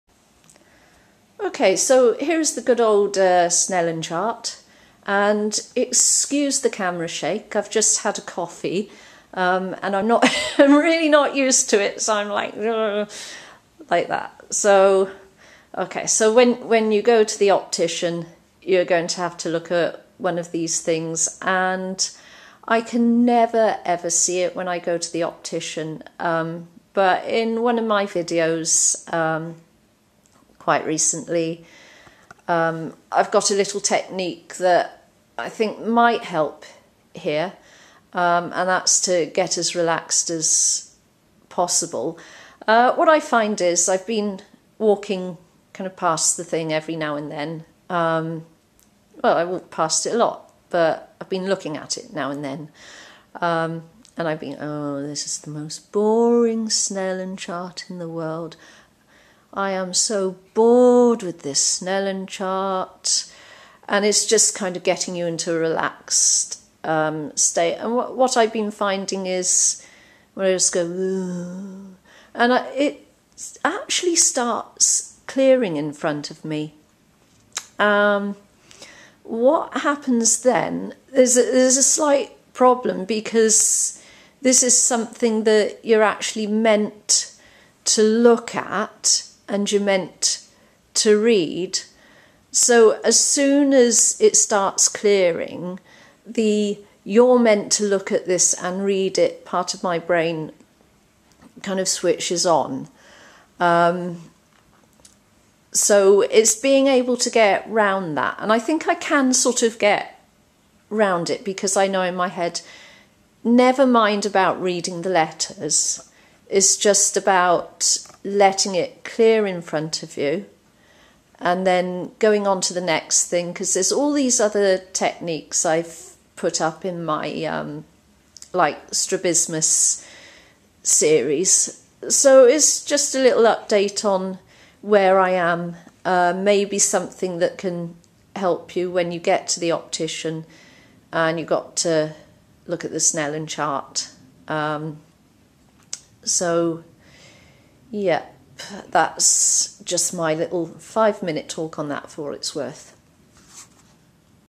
Snellen chart
An update on how to imrove your Snellen score ednmyopia